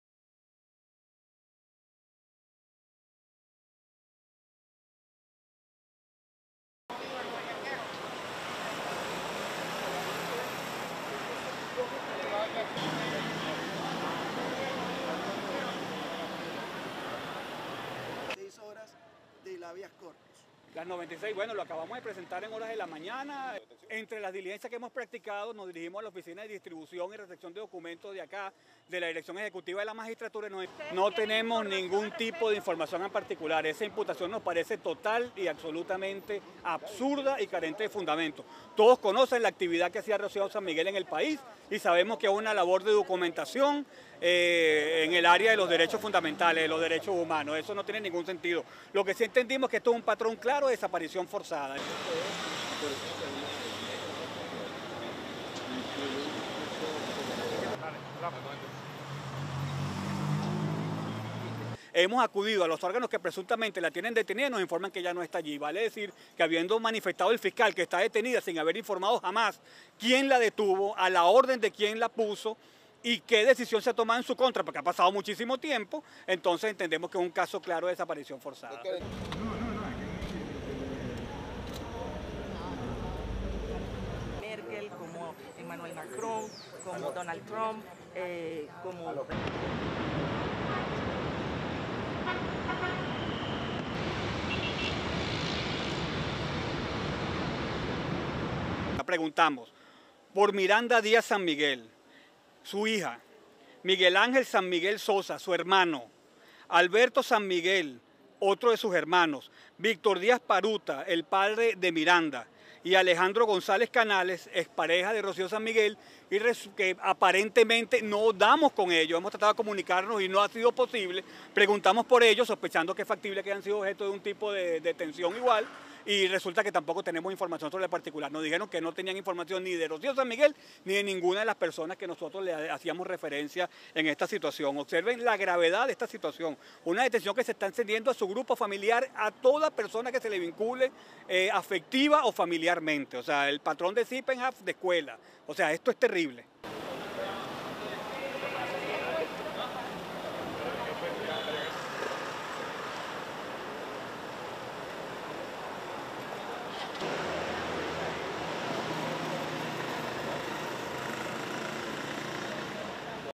Whereabouts of Venezuelan human rights attorney and her family remain unknown days after detentions
RESTRICTION SUMMARY:

ASSOCIATED PRESS
ARCHIVE: Caracas, Venezuela -  14 September 2011
1. STILL: Venezuelan activist Rocio San Miguel, who leads a private organization focused on national security and defense issues, talks during a conference in Caracas

ASSOCIATED PRESS
Caracas, Venezuela - 12 February 2024
2. Various of outskirts of Justice Palace
3. Various of defense lawyer of Rocío San Miguel, Juan Gonzalez, speaking to the press
4. SOUNDBITE (Spanish) Juan Gonzalez, lawyer:
"We do not have any particular information. This accusation seems to us totally and absolutely absurd and unfounded. Everyone knows the activity that Rocío San Miguel carried out in the country and we know that it is a work of documenting fundamental rights, human rights. This does not make any sense. What we do understand is that it is a clear pattern of forced disappearance."
5. Gonzalez looking at his phone
6. SOUNDBITE (Spanish) Juan Gonzalez, Rocío San Miguel's lawyer:
"We have gone to the agencies (Bolivarian Service of Intelligence SEBIN and General Direction of Military Counter Intelligence DGCIM), that are supposedly holding her in custody and they inform us that she is not there. It is worth saying that the Attorney General has stated that she is detained without ever informing us who detained her, to which authority she was taken to, and what is the ruling against her, because a very long time has already passed.  So we understand that this is a clear case of forced disappearance."

ASSOCIATED PRESS
ARCHIVE: Caracas, Venezuela - 20 February 2020
++ NIGHT SHOTS ++
7. Various of police vehicle from the General Direction of Military Counter Intelligence (DGCIM)

ASSOCIATED PRESS
ARCHIVE: Caracas, Venezuela - 12 September 2022
8. Various of Bolivarian Service of Intelligence (SEBIN) prison known as "The Helicoid" headquarters

ASSOCIATED PRESS
9. SOUNDBITE (Spanish) Juan Gonzalez, Rocío San Miguel's lawyer:
"We ask for Miranda Díaz San Miguel, her daughter; Miguel Ángel San Miguel Sosa, her brother; Alberto San Miguel, another of her brothers; Víctor Díaz Parota, Miranda's father and Alejandro González Canales, Rocío San Miguel's ex-partner. Apparently we can't find them, we have tried to communicate with them and it has not been possible. We asked about them (at intelligence agencies), suspecting that it is feasible that they have been subject to a similar type of detention and it turns out that we have no information on the matter either. They told us that they had no information about Rocío San Miguel or any of the people we referred to in this situation. Note the seriousness of this situation, a detention that is being extended to her family group, to any person who is related affectively or by kinship to her. This is a clear (case of) Sippenhaft pattern. This is terrible."
10. Various of headquarters of Justice Palace building
STORYLINE:
The whereabouts of a prominent human rights attorney, her daughter and ex-husband remained unknown Monday, three days after Venezuelan authorities detained her at the airport near the capital as she and her adult child awaited a flight to Miami, setting off condemnation inside and outside the South American country.

Attorneys for lawyer and activist Rocío San Miguel, Miranda Díaz San Miguel and Victor Díaz Paruta could not locate their clients Monday at detention facilities in Caracas despite confirmation Sunday from Attorney General Tarek William Saab of San Miguel’s detention.

Two brothers of San Miguel also went missing after she was taken into custody and are presumed detained.